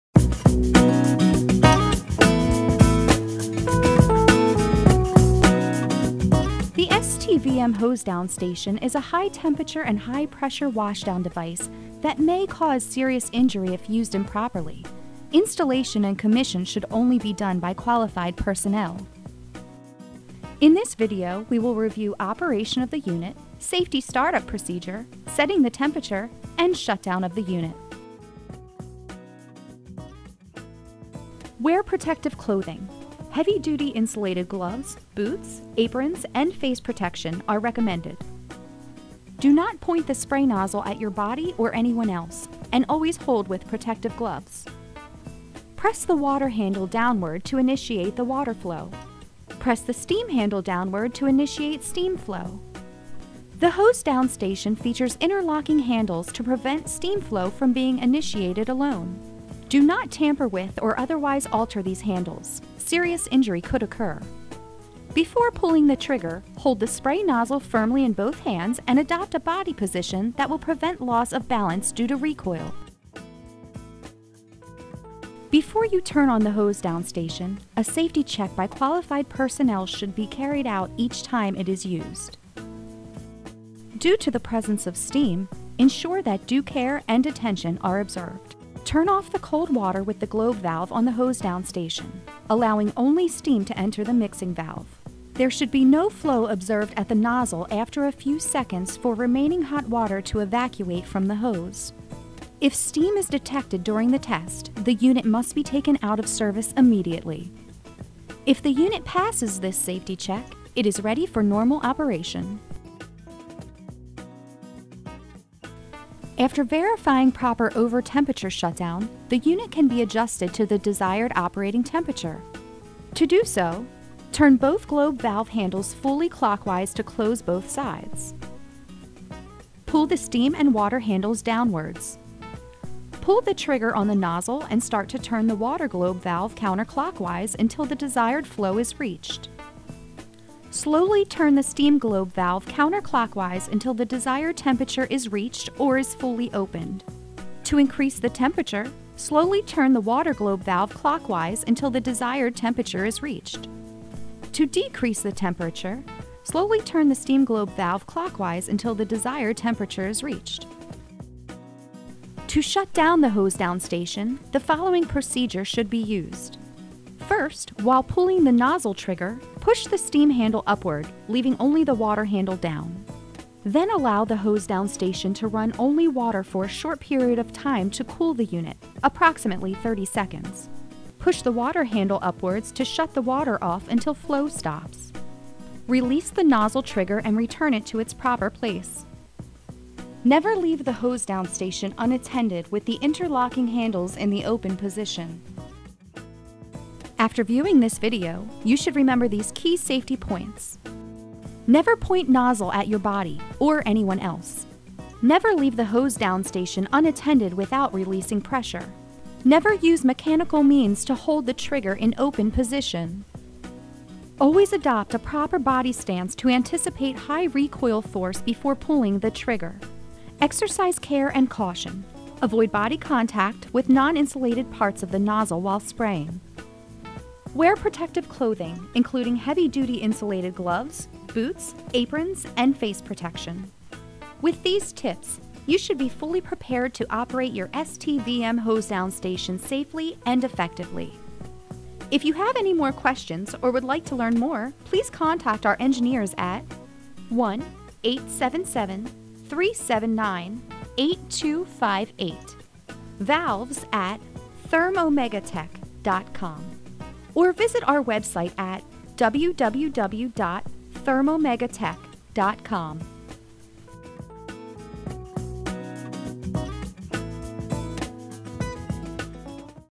ThermOmegaTech's STVM® Washdown Station Operation
How to operate Therm-Omega-Tech's STVM® Washdown Station properly and safely.
In this video we visually guide you through operation, safe usage, and proper shutdown procedure.